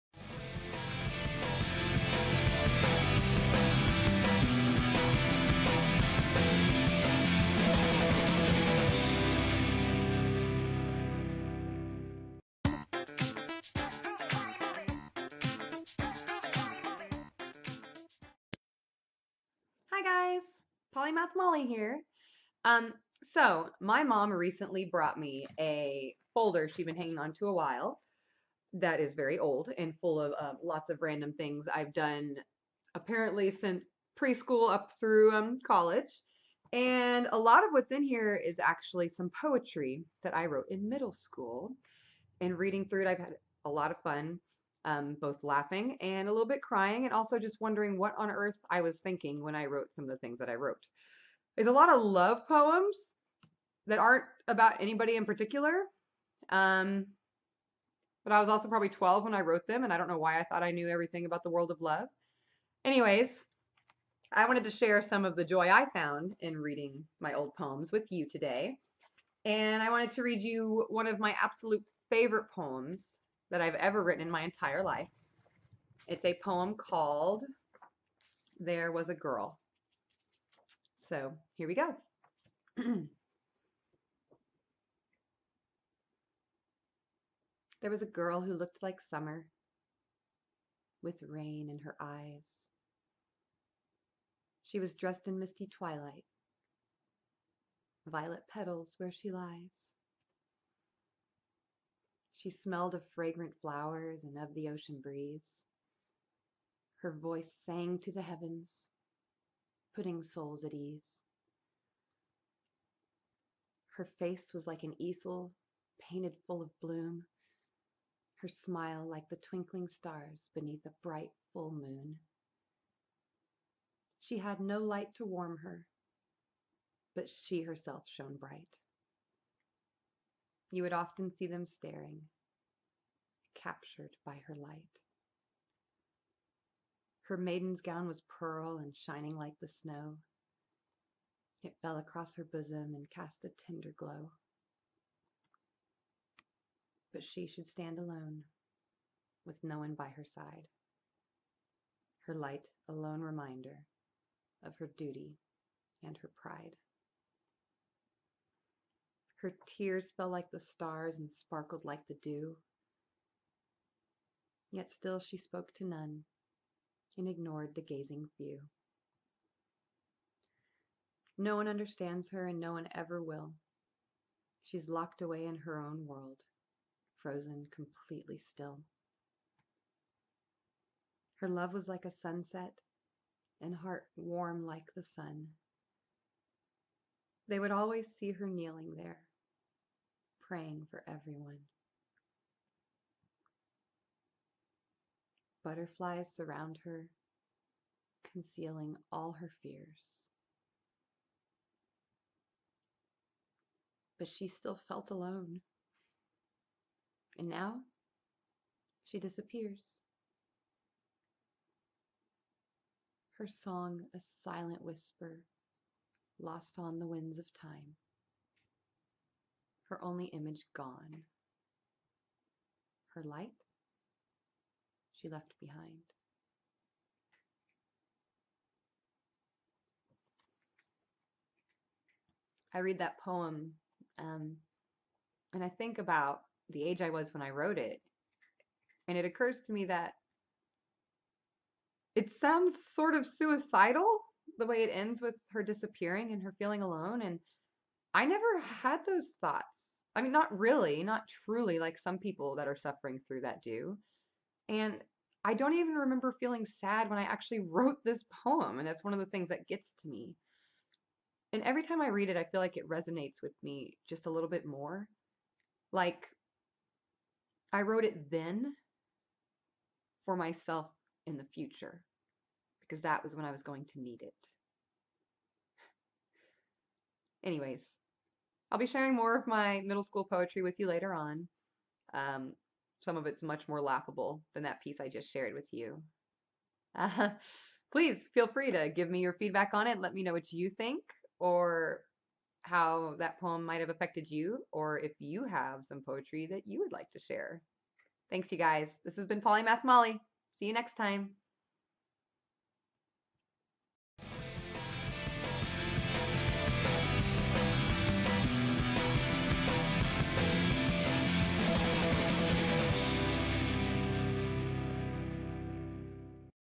Polymath Poet ~ There was a Girl. . .
A preview of the kind of poet I was in 7th grade. Please enjoy a taste of my teenage angst! XD